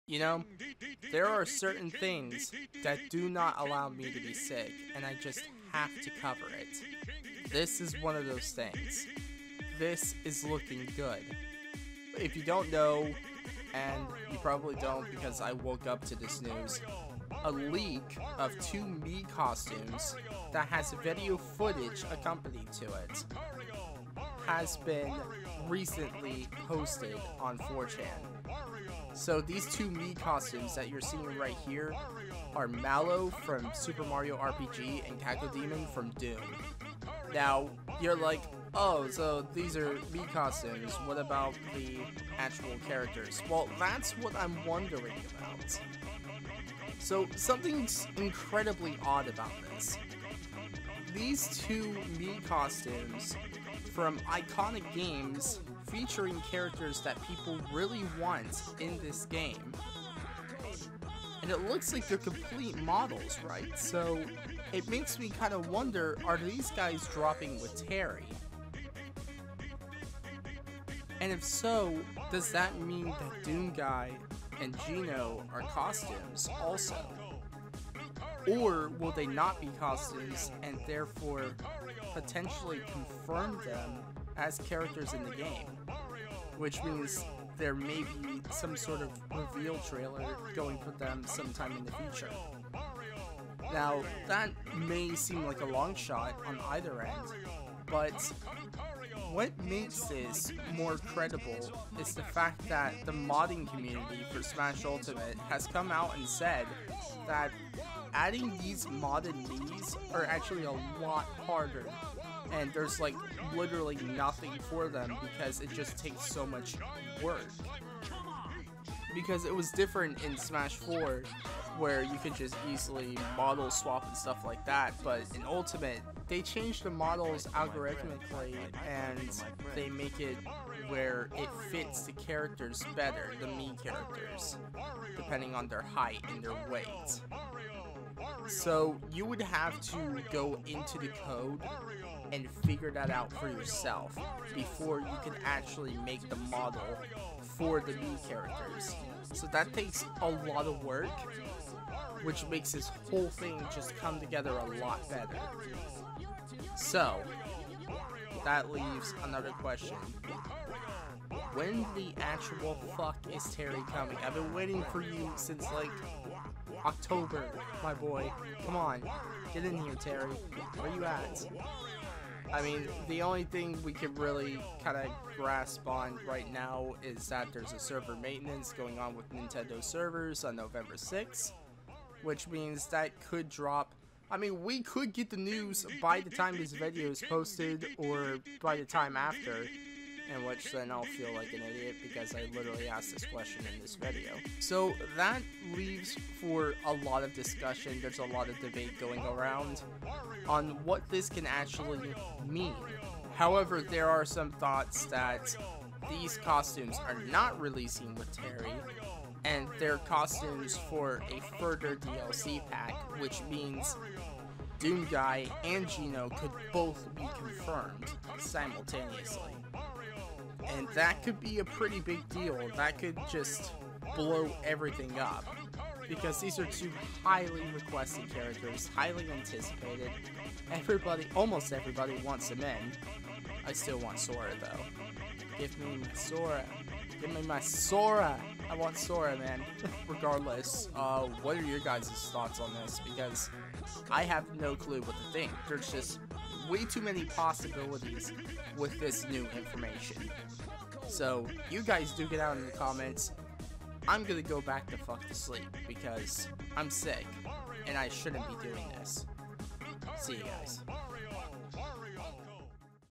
VIDEO LEAK OF FUTURE MII COSTUMES?
A video on 4chan might have just revealed some Mii costumes, which in turn could mean some highly requested characters getting into the game.

Some important sources